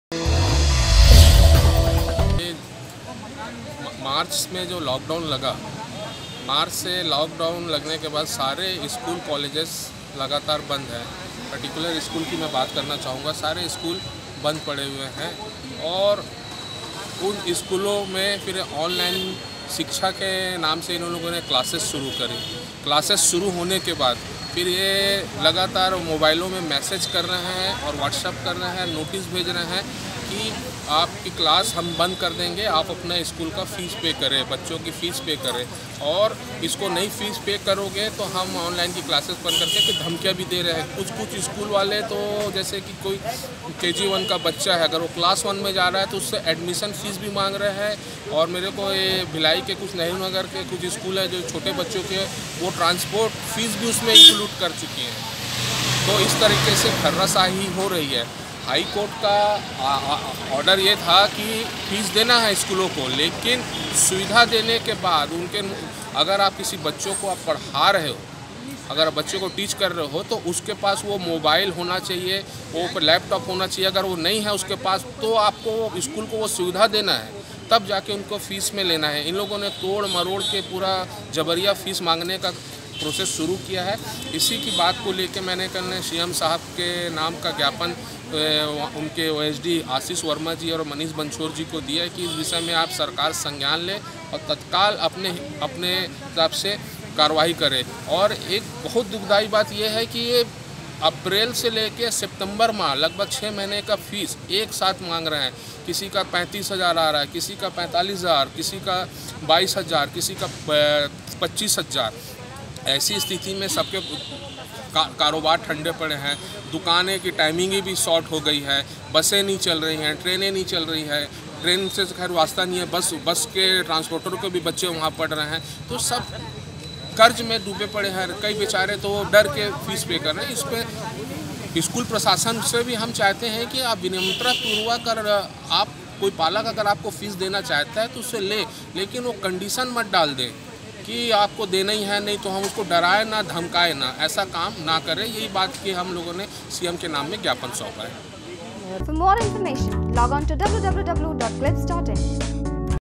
फ़ीस की मांग को रोकने के लिए युवा कांग्रेस ने मुख्यमंत्री के नाम सौंपा ज्ञापन
दुर्ग। युवा कांग्रेस ने निजी स्कूलों के फीस की मांग को अनुचित बताया है और इस संबंध में मुख्यमंत्री के नाम ज्ञापन सौंपा है। युवा नेता अय्यूब खान ने बताया कि निजी स्कूलों के संचालक अप्रैल से सितंबर माह तक कि फीस को एक साथ जमा करने की मांग पालकों से व्हाट्सऐप और एसएमएस भेजकर कर रहे हैं।